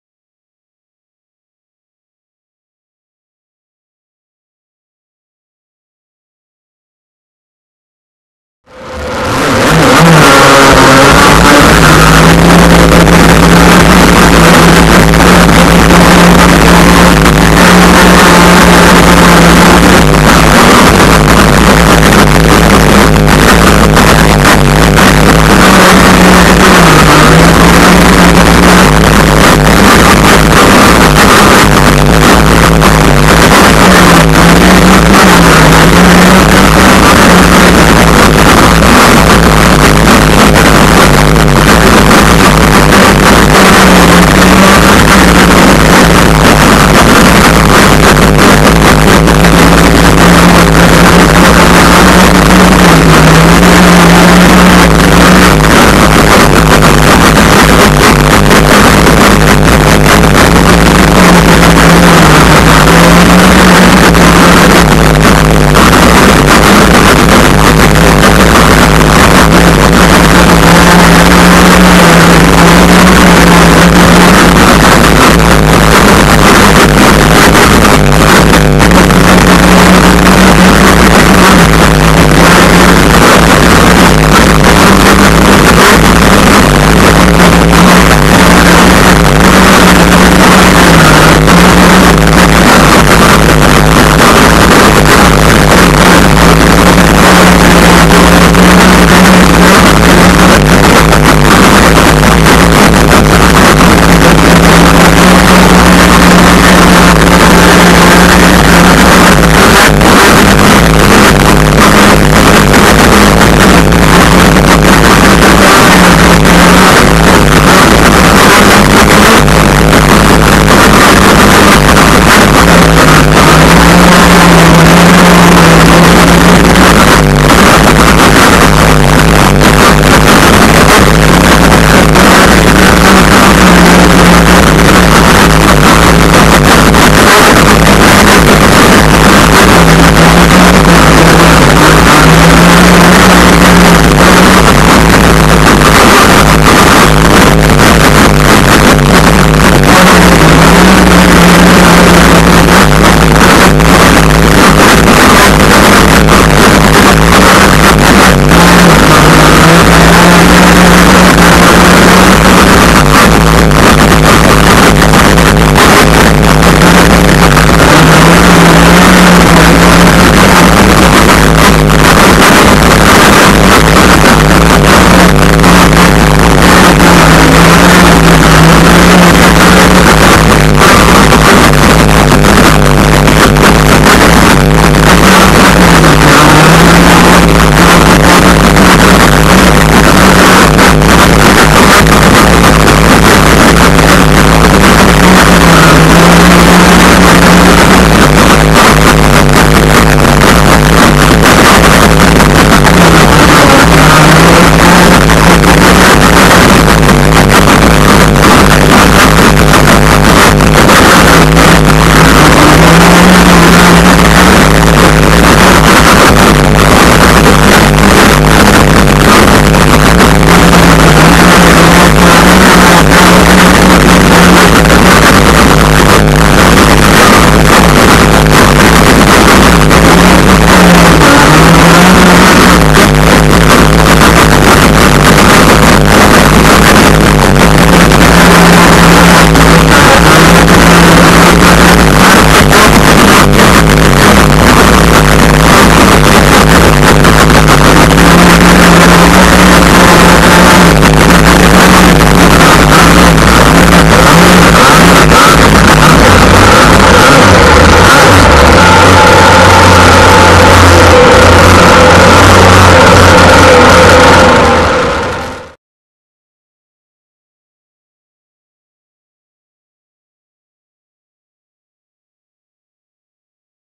Enforcer Crackerbox Rc Boat Onboard Prop Testing
Trial and Error will teach you. The Enforcer Crackerbox will get a variety of props tested on it to see what works best. Pond time is irreplaceable and I’m learning what works and what don’t. It’s all part of the Fun. Come onboard for a few laps and Enjoy! Take Care and Stay Safe Everyone!